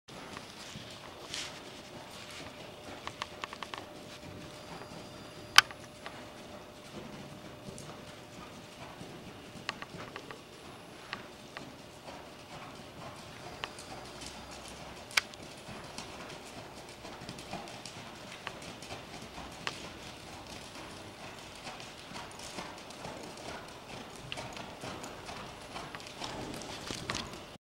2003 AQHA HUS Grey Gelding - Heza Wynning Style Jog Demo 1
Heza Wynning Style shown jogging. Currently in training at Peak Performance Horses, Hebron, Indiana. $5,500.